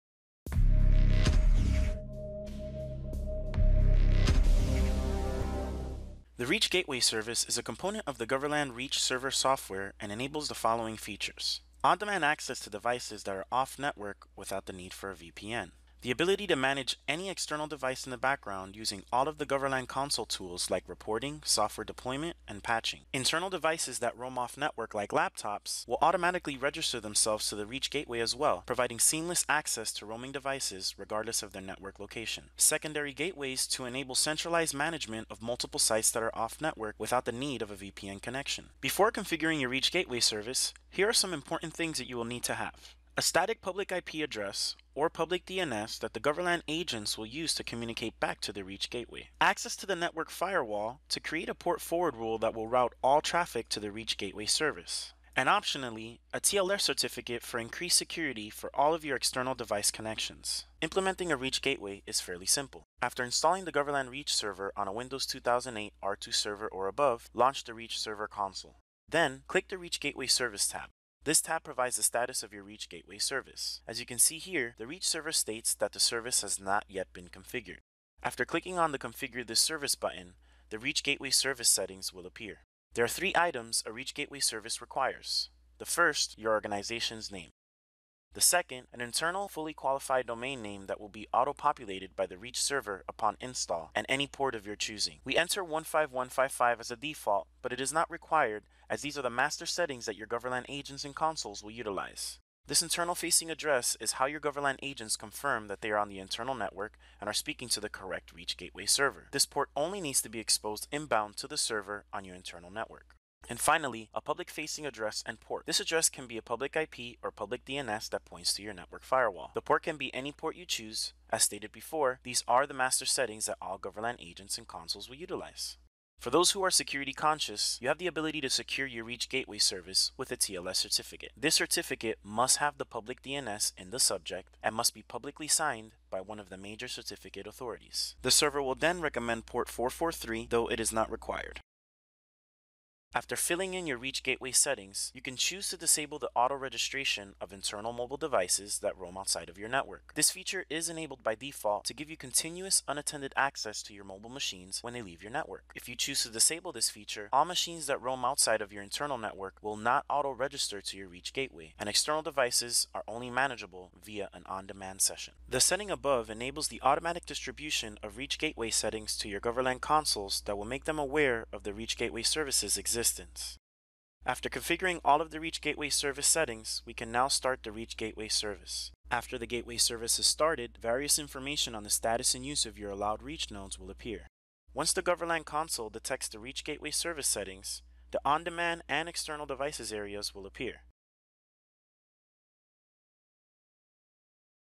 Reach Gateway Service - Implementation - Goverlan Reach
Learn how to setup the Goverlan Reach Gateway Service on the Goverlan Reach Server, which enables External Device Management.